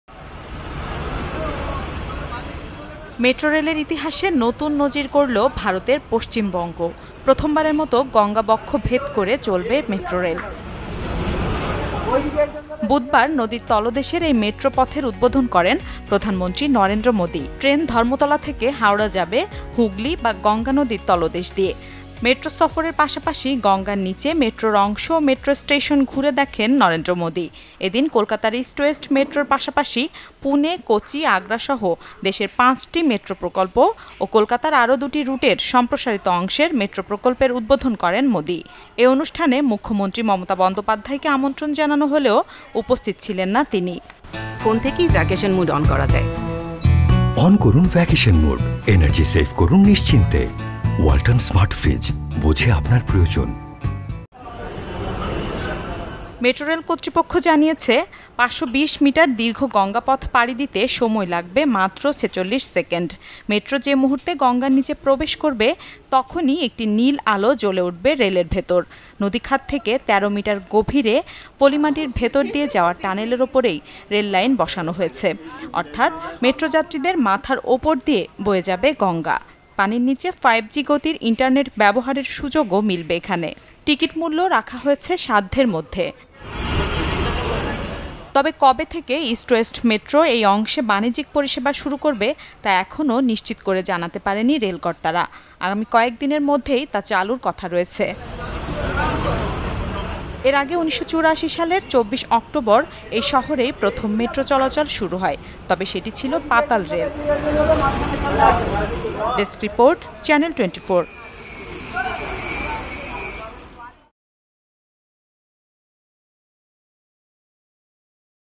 নদীর নিচ দিয়ে ছুটে চলছে মেট্রোরেল | New Metro Rail | Kolkata | India | Channel 24
»» Office Address:
Bangladesh.